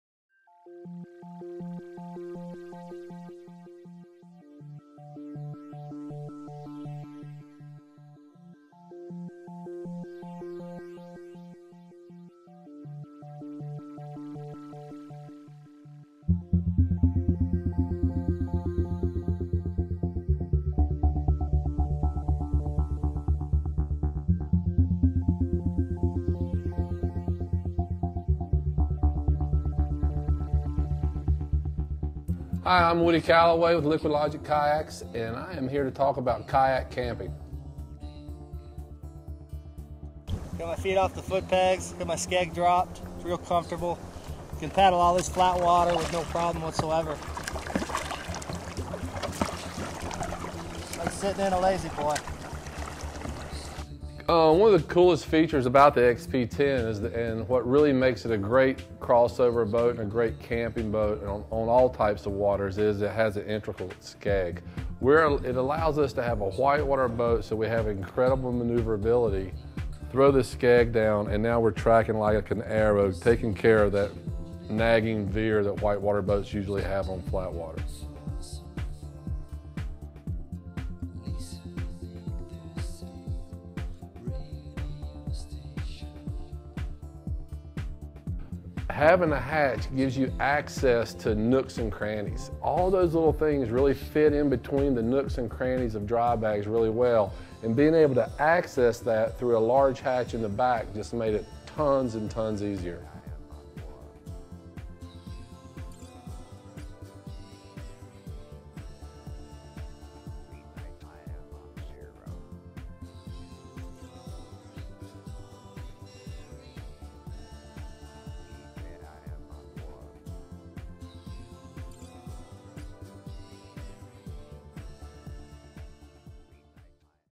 camping_XP_boat.mp4
SomeWoody talks about selecting a boat for Kayak Camping.